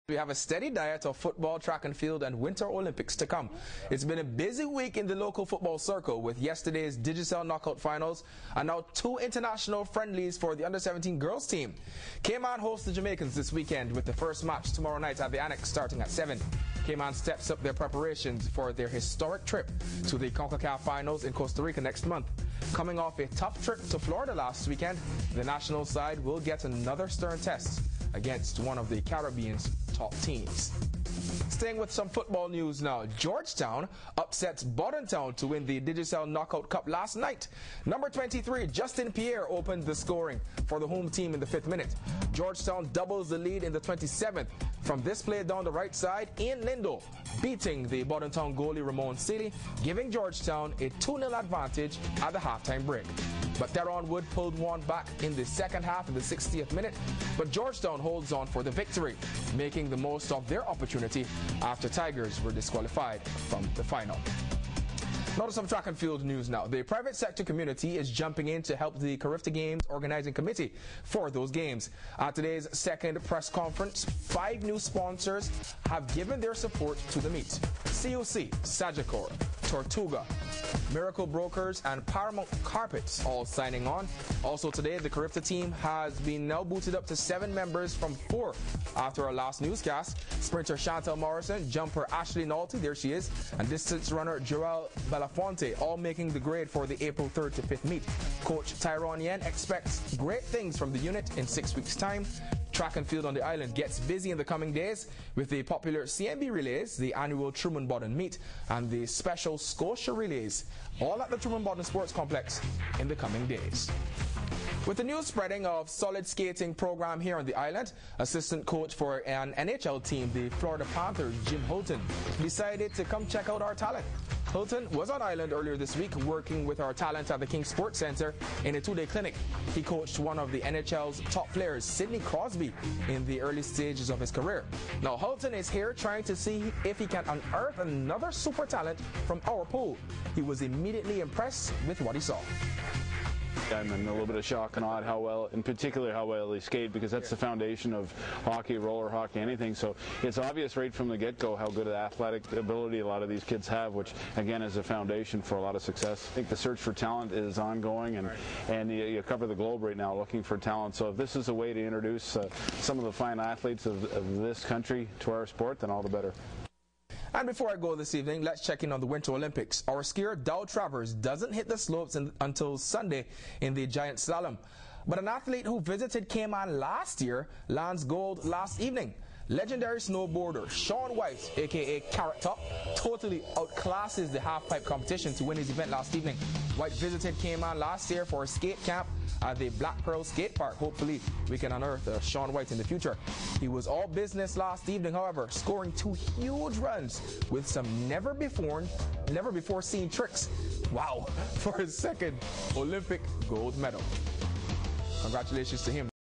Feb 18th Sports News from Cayman27
Cayman Football: U17 Girls prepare for Jamaica and George Town FC beat Bodden Town FC to the Digicel Cup. Cayman Athletics: CARIFTA getting closer. Cayman Roller Hockey: Coaching Clinic